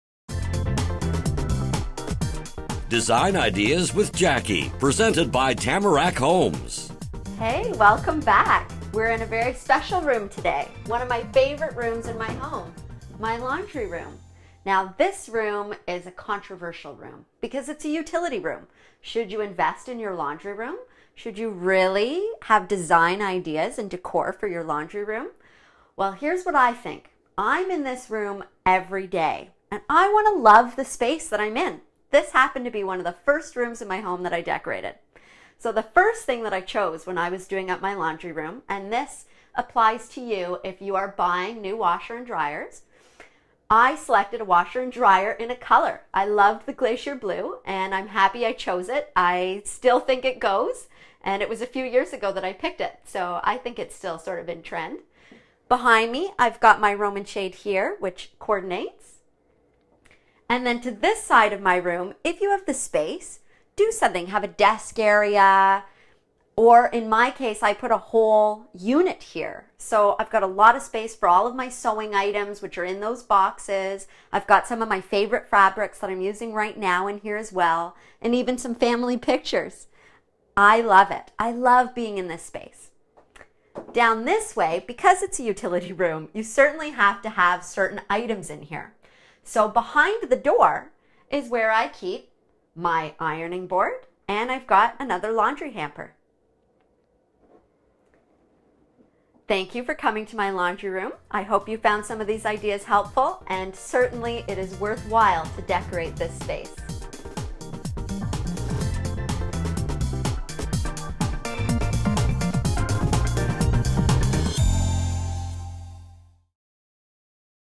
Tamarack Design Ideas 37
Laundry Rooms can be overlooked- Jacqui shares with us why she didn't overlook adding design and colour to her own laundry room! Jacqui, can we come fold some laundry in there? It's beautiful!